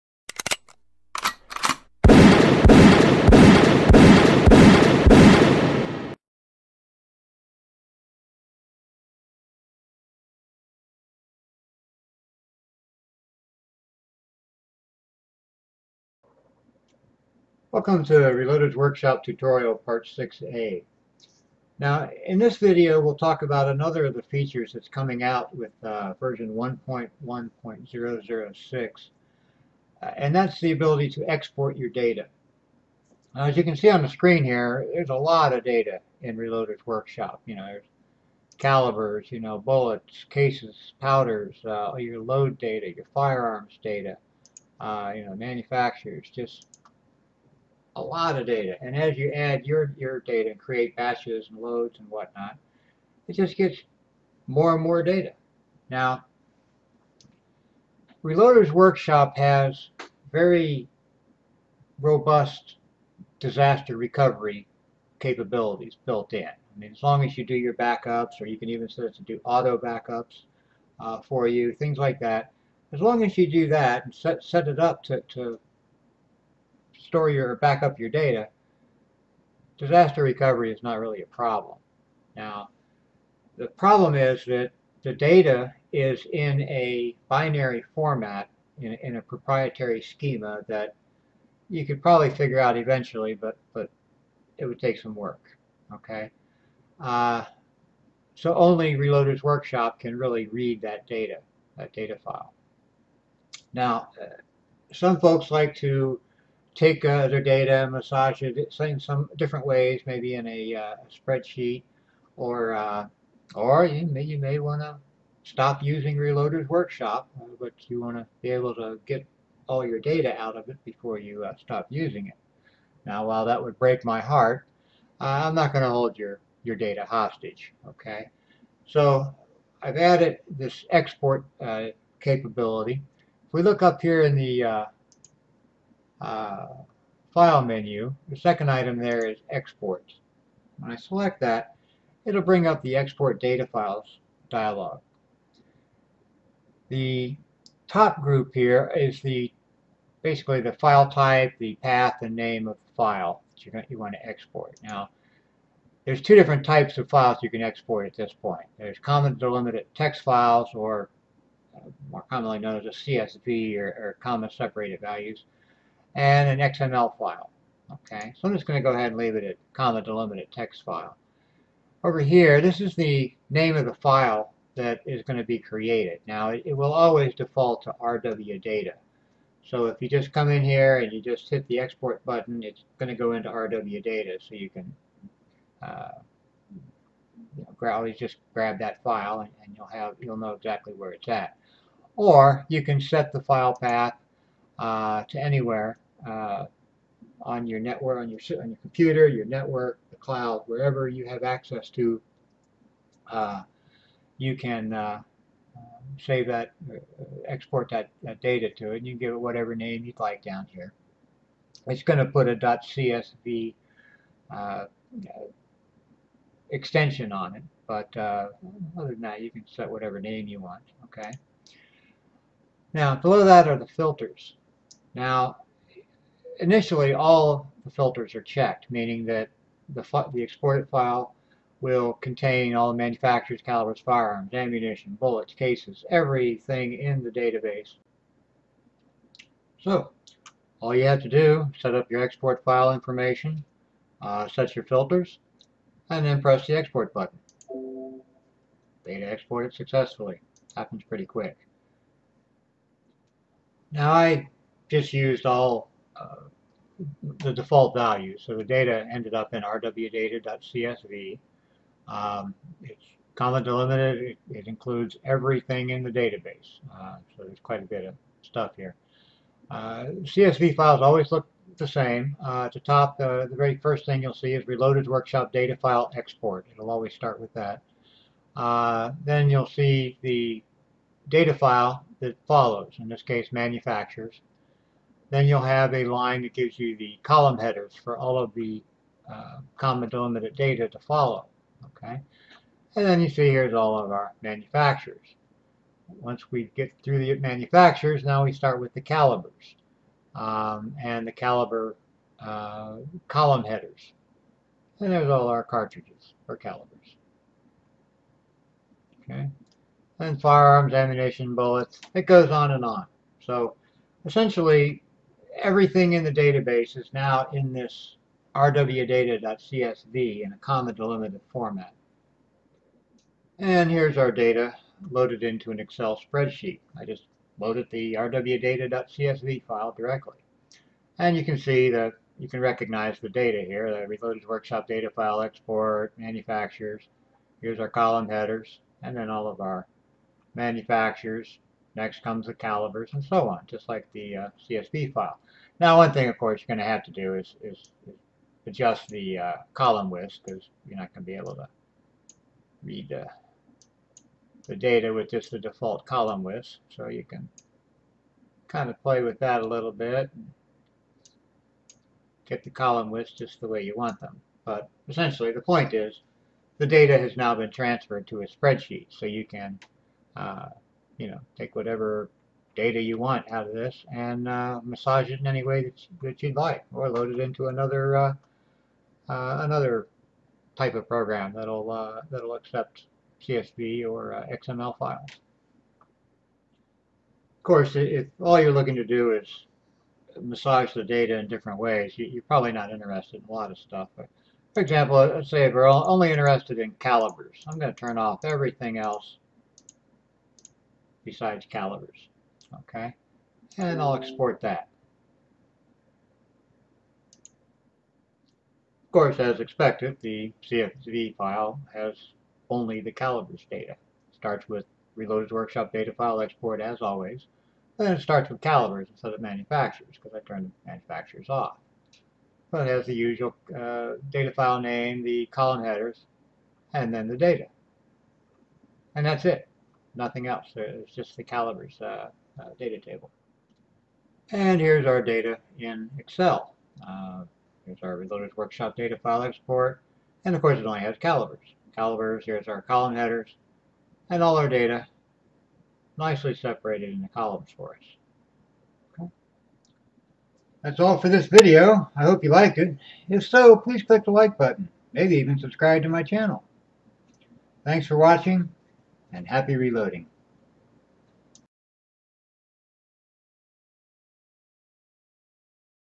Reloader's WorkShop Tutorial - Part 6a - Exporting Data
Another feature in Version 1.1.006 will  be the ability to export your data to a csv or xml file.